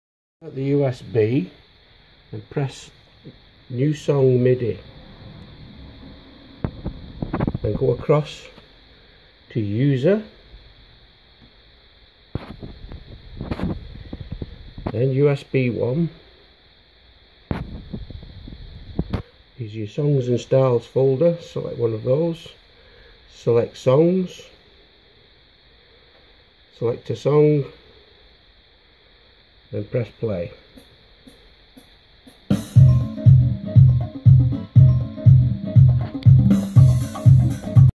Loading a midi file (Song) on the PSR SX 900 and 700
Quick video showing how to load a midi file (song) on the PSR SX range of keyboards